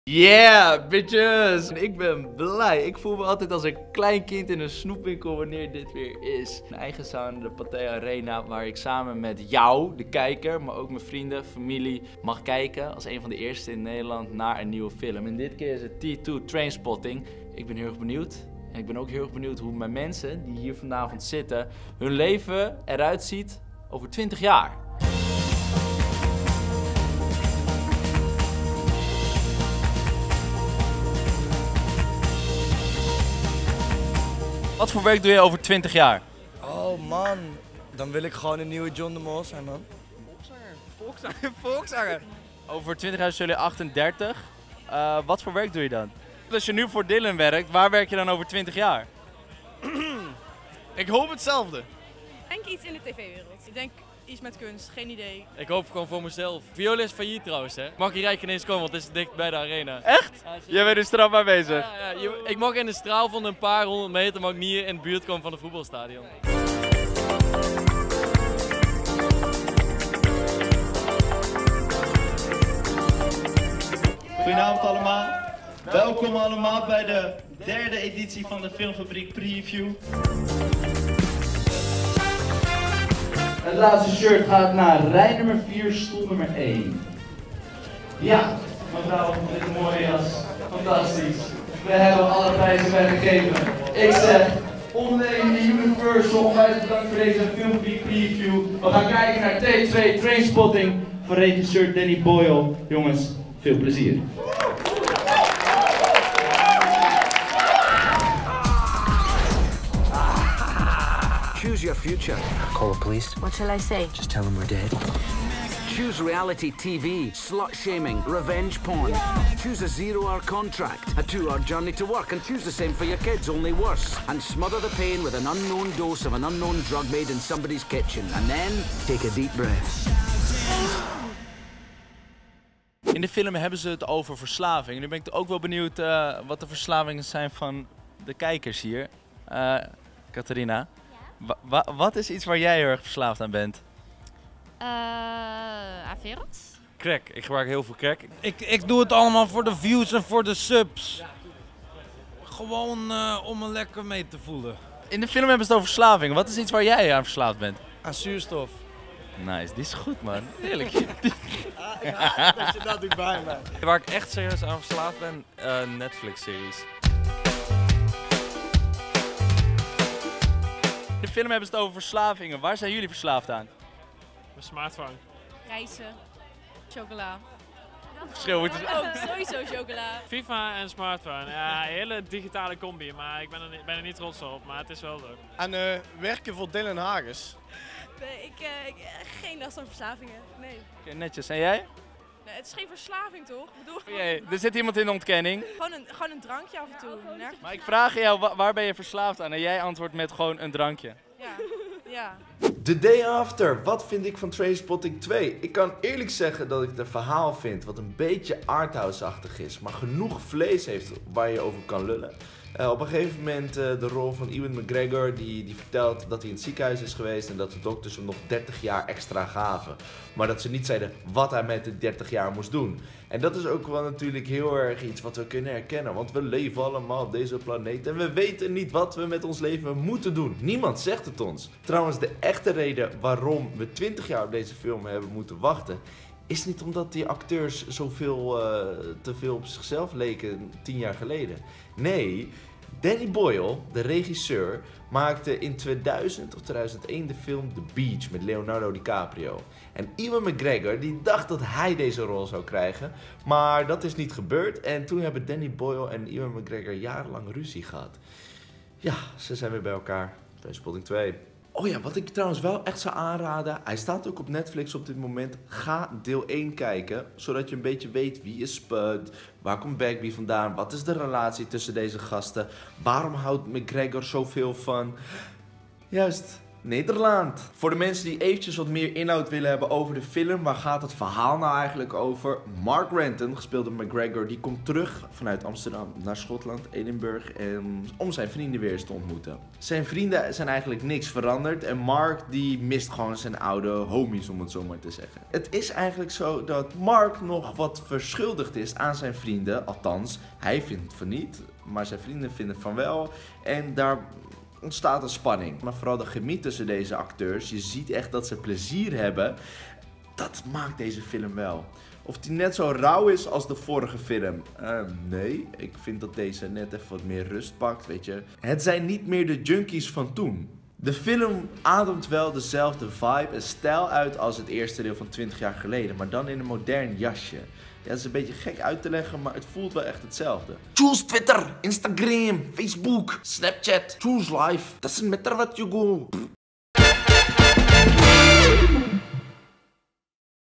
T2 TRAINSPOTTING (2017) - FilmReview
Yo FilmBuddies! In deze video geef ik je een blik achter de schermen tijdens m'n FilmFabriek Preview, T2 Trainspotting! Daarnaast vertel ik je ook wat ik van de nieuwe film van Danny Boyle vind.

Synopsis:
Twintig jaren zijn verstreken. Er is veel veranderd maar er is ook veel hetzelfde gebleven. Mark Renton (Ewan McGregor) keert terug naar de enige plek die hij zijn thuis kan noemen. Daar wachten ze op hem: Spud (Ewen Bremner), Sick Boy (Jonny Lee Miller), en Begbie (Robert Carlyle) die van Mark nog wat geld krijgen...

Cast & Regie:
Danny Boyle: Regisseur
Ewan McGregor: Mark Renton
Robert Carlyle: Begbie
Ewen Bremner: Spud
Jonny Lee Miller: Simon

Bedankt Universal Pictures Nederland en OnLane bij het mee organiseren van deze exclusieve FilmFabriek Preview!

Meer FilmFabriek?